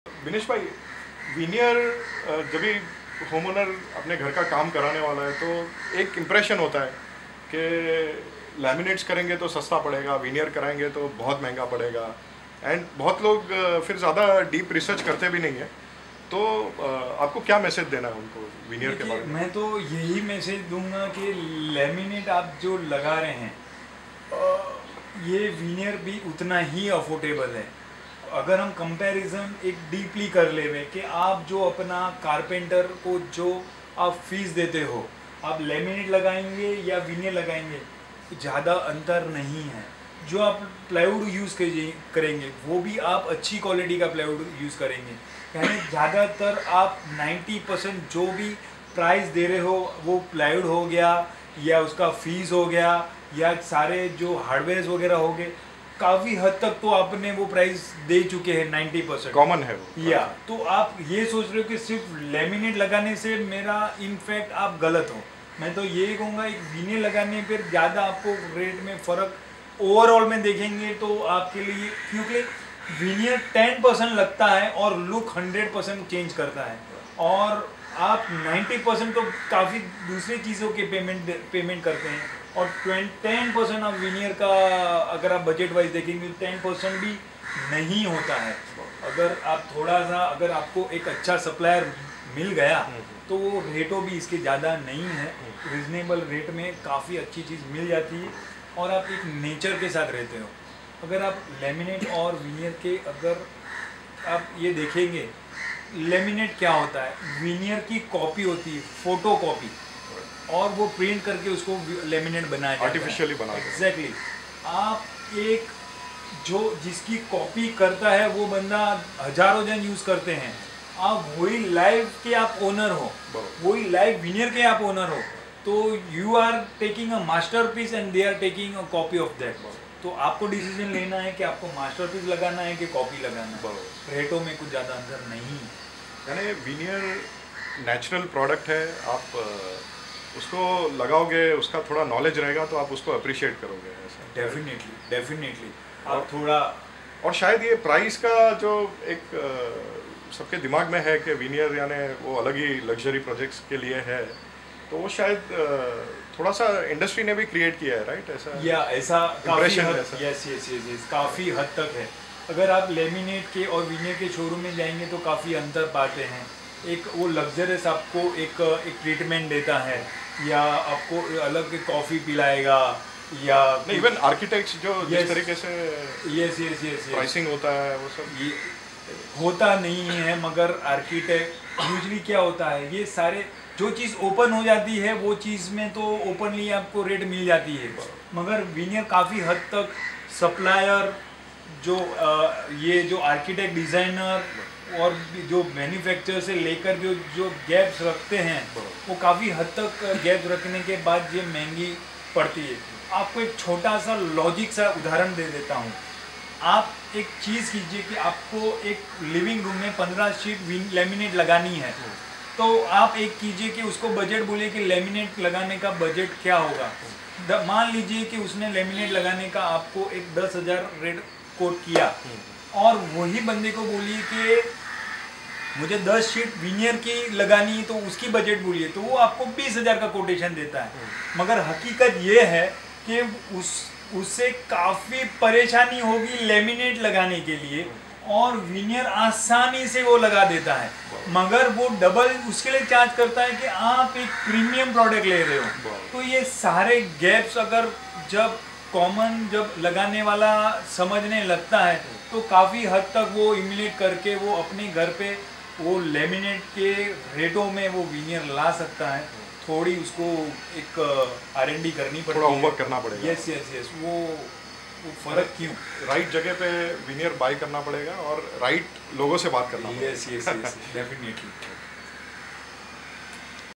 Veneer vs Laminate
Veneer v/s Laminate

Nitin: What do you have to say for home owners who think that veneer furniture’s are quite expensive than laminate made furniture’s.
Binesh Shah: Veneers today are as affordable as laminates. Even if one goes for hard core comparison, they will fail to find one.
The plywood used to make furniture will definitely be of good quality. Truly speaking 90% of the cost for making furniture includes Plywood cost+ labor charges+hardware. The remaining 10% cost is either Laminate or Veneer cost. You just paying 10% of budget for veneer or laminate, but only veneer delivers 100% good result. I would say 10% cost for veneers is quite a lot sometimes. Veneers are available for good affordable rates too. With Veneer you feel next to nature.
Whereas Laminates are an artificial product that are copy of veneers you can say. You therefore get to see varied number of copies and styles of veneers in Laminates.
Having Veneer is like having a masterpiece while buy having laminates you take away the copy of nature. Well cost-wise again, here’s no much of a difference.
Nitin: Buyers who know and understand the importance of veneer will definitely appreciate it. But hasn’t the industry hyped Veneer as luxury product?
Binesh Shah: Yes. Yes. One can feel the difference by just visiting Laminate and Veneer show room differently. You will receive very luxurious treatment when inside veneer showroom. Many Architects are responsible to create Veneers as Luxury product impression especially cost-wise. Cost-wise, I would repeat that there is no much of a difference. It’s easy to find out rates for products that are locally and easily available. With Veneer, there major cost hidden right from manufacturer to supplier which makes Veneer a costly product.
For eg: A professional may quote 10,000/- for 15 laminate sheet however for 10 veneer sheets he will quote 20,000/-. Whereas in reality there’s not much of cost difference between the two. He is charging you more for having a premium product- Veneer. Laminate if you see are more difficult to work with than veneers.
With correct know-how, right people to guide and little of R&D, home owners can easily buy Veneers at right price and feel next to nature.